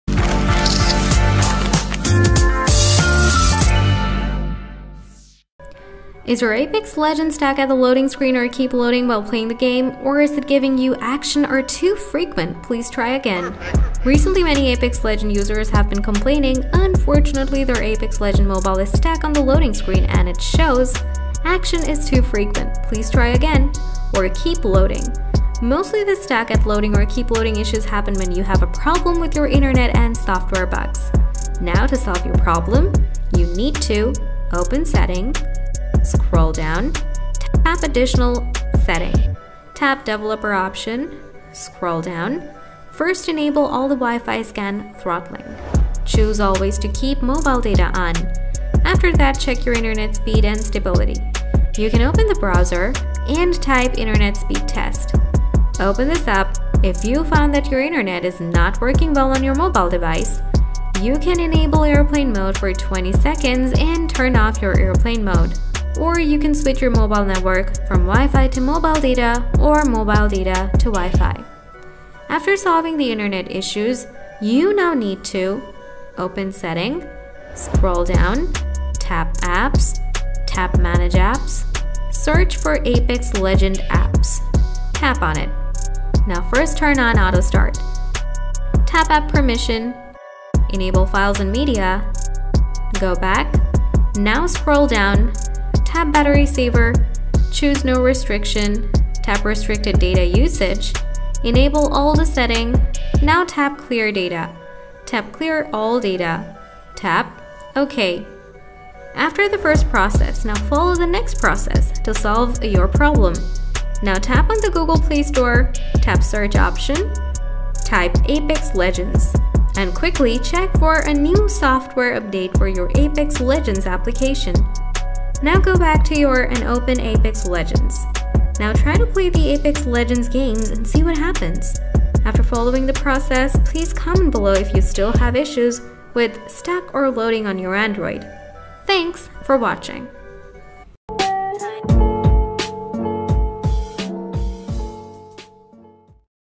How To Fix Apex Legends Stuck at Loading Screen | Keep Loading Issues on Apex Legends
Is your Apex legend stuck at the loading screen or keep loading while playing the game? To fix apex legends stuck at loading screen, watch this video.
Recently, Many Apex legends users have been complaining that Apex legends stuck on loading screen and they want to fix keep loading issue on apex legends. So if you are having loading screen problem in Apex legends or stuck at keep loading issues then don't worry!
So after analyzing the fact, I will show you how to fix apex legends stuck at loading screen or Apex Legends game not opening on Android.

00:01- Video intro
00:33- Solution 1: Enable some settings from the developer option
00:50- Solution 2: Check your internet and fix it
01:15- Solution 3: Remove software bugs
01:55- Solution 4: Check for a new software update
02:20- Solution ending